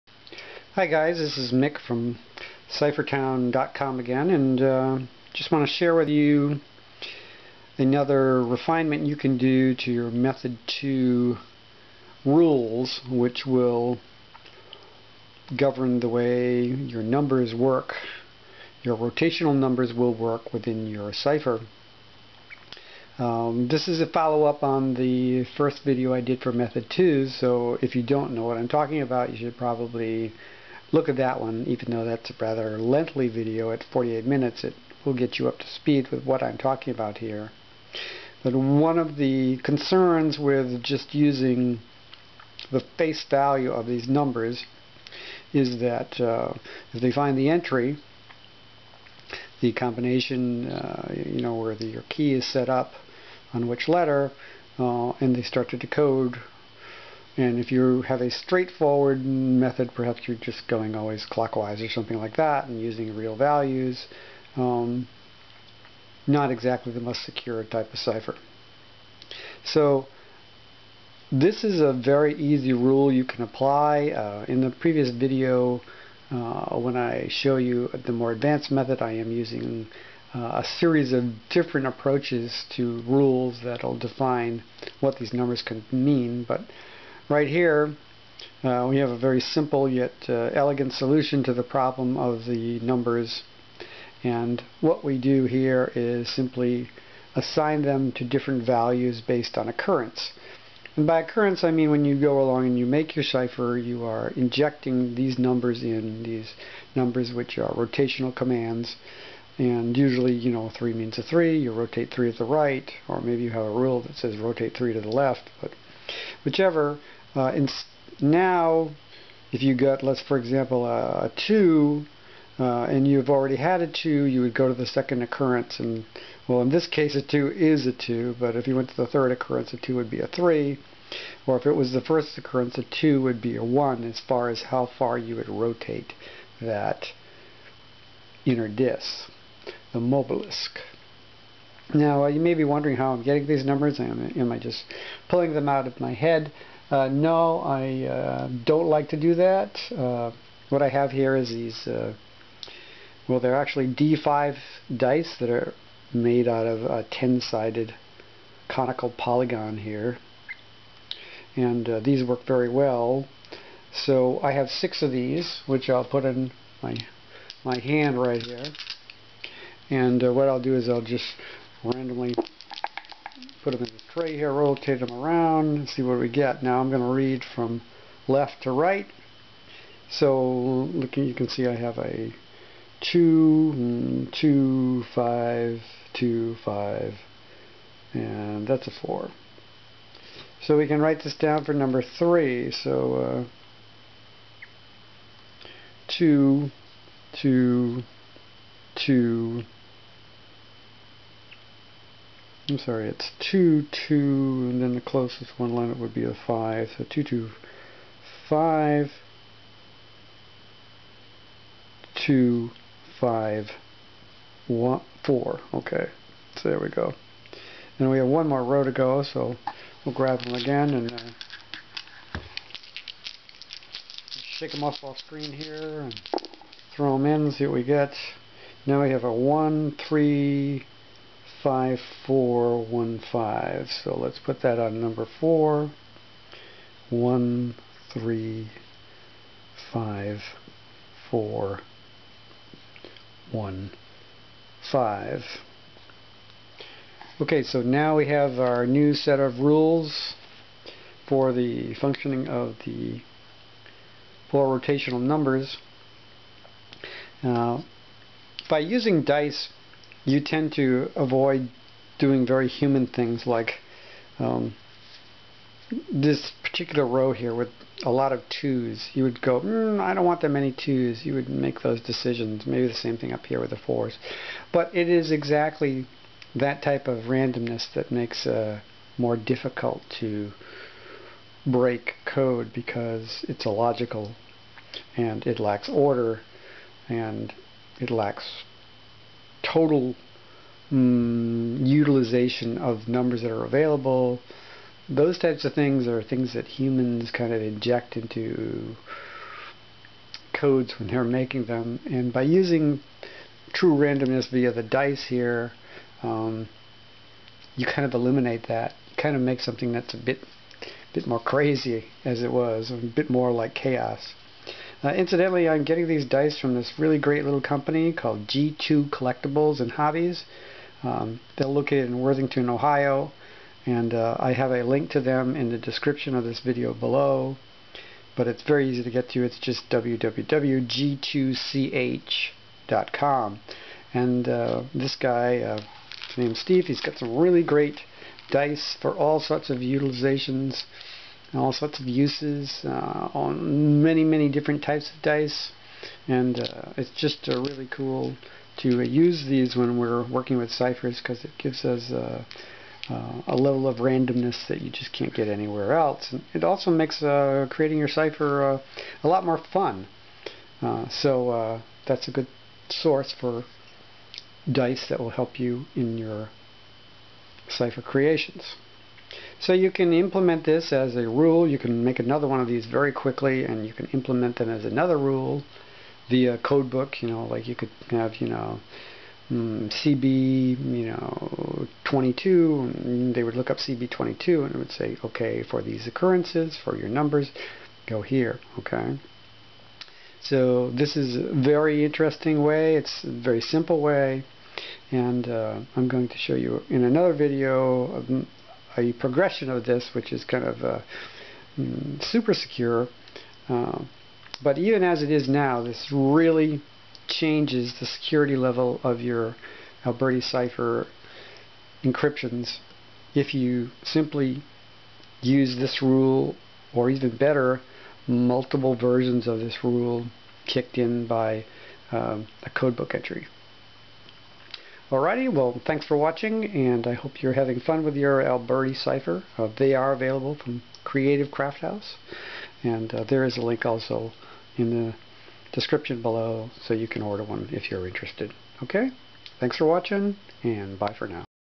Alberti Cipher Disk Encoding w/Method 2, Rules for Changing Rotational Command Values
In this brief video, we discuss employing a rule to change the default values of the rotational numbers 1 thru 4, as used with Alberti's 2nd Encryption Method.